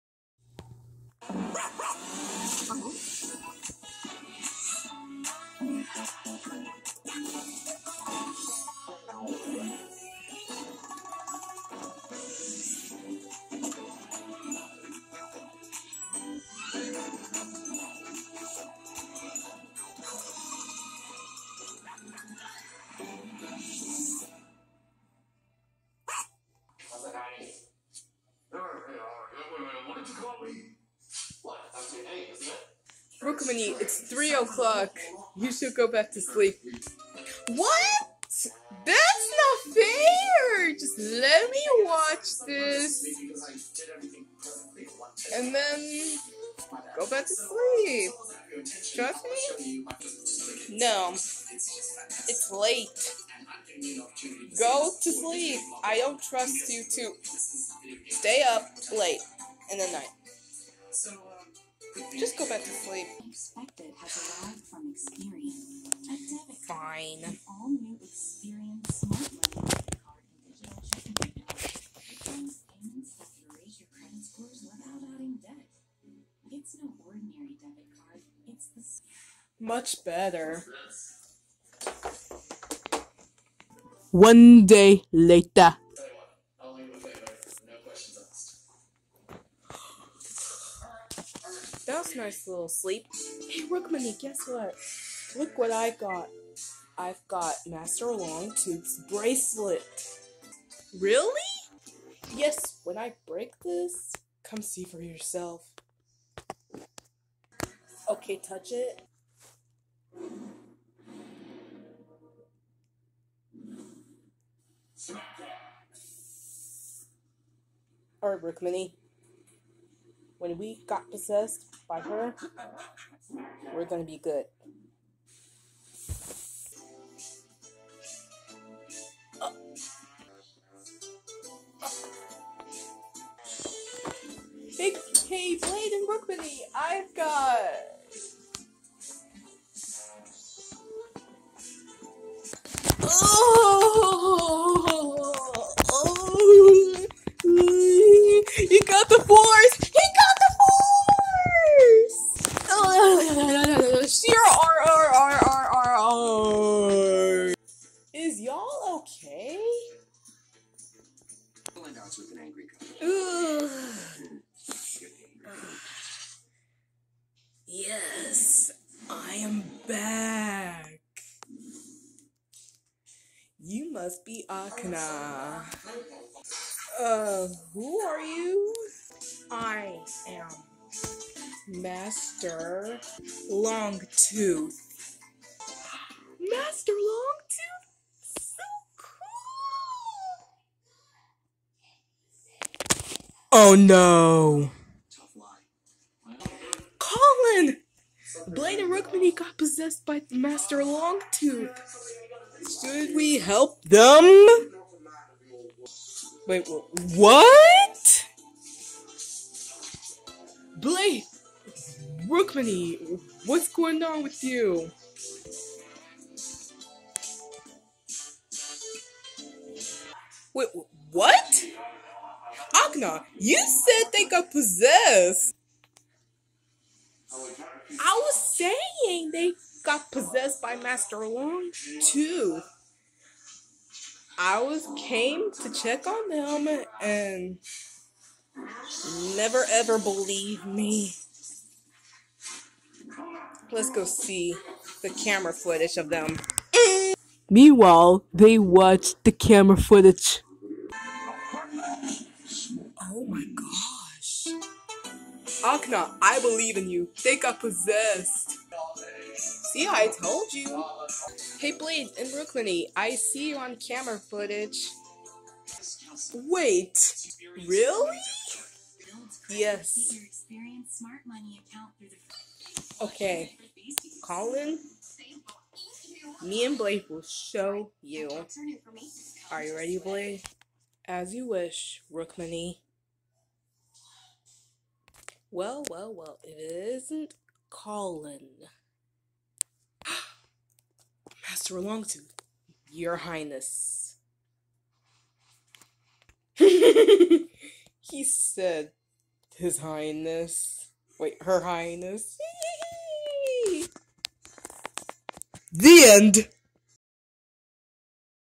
Blade and Rukhmini get Possessed by Master Longtooth (Kung Fu Panda: The Dragon Knight Parody)
I Got a New Intro! I forgot to Set the Outro up later.